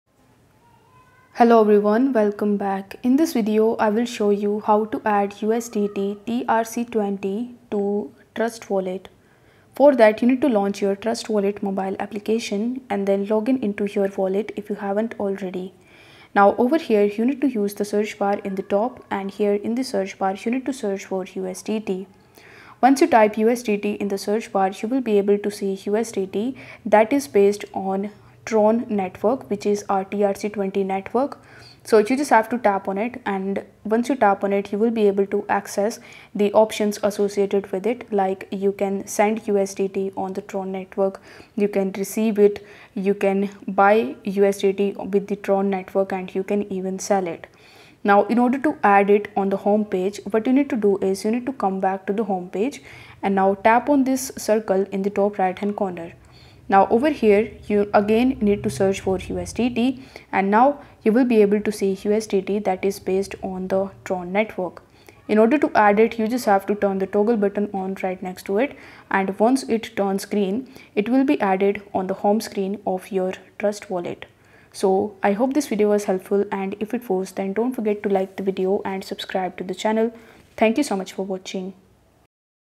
How to Add USDT TRC 20 to Trust Wallet IN 20 SECONDS!
Welcome to Token Tutorials! In this video, I will show you How to Add USDT TRC 20 to Trust Wallet IN 20 SECONDS!